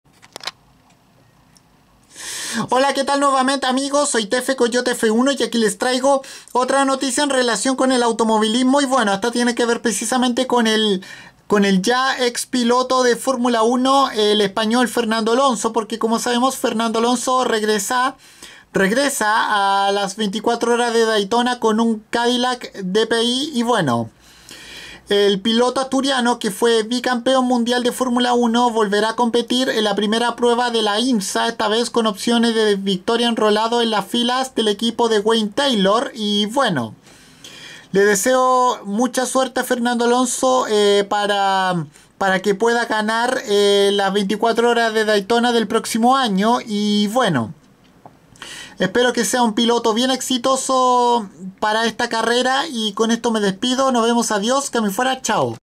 Fernando Alonso volverá a las 24 Horas de Daytona con un Cadillac DPI
El asturiano bi campéon mundial de F1 esta vez tendrá como objetivo la victoria en su segunda participación en la famosa prueba de la IMSA